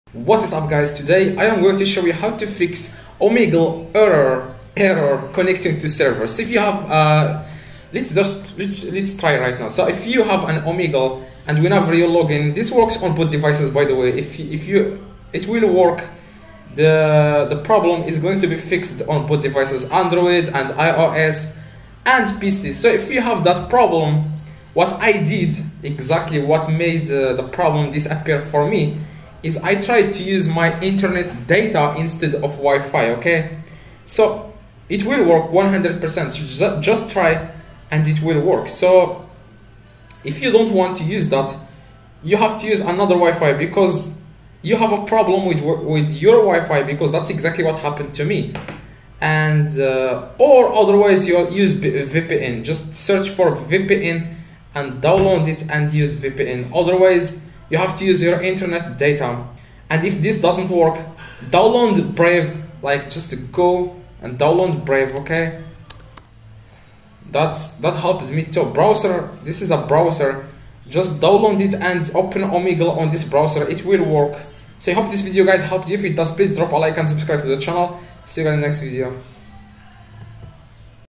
How to Fix Omegle Error Connecting to Server (Updated Guide)
in this video, I'll be showing you how to fix  Omegle Error Connecting to Server
The Omegle error connecting to server can happen due to many reasons.
First, try using the website on another device in case the issue comes from their end
Since your network might be at fault, try connecting from another one.
You can also use a reliable VPN that will not get blocked by the website.
If you’re a long-time Omegle user, you might be familiar with the Omegle error connecting to the server issue.
However, you can get rid of this error message by watching the video
drop a like for good luck:)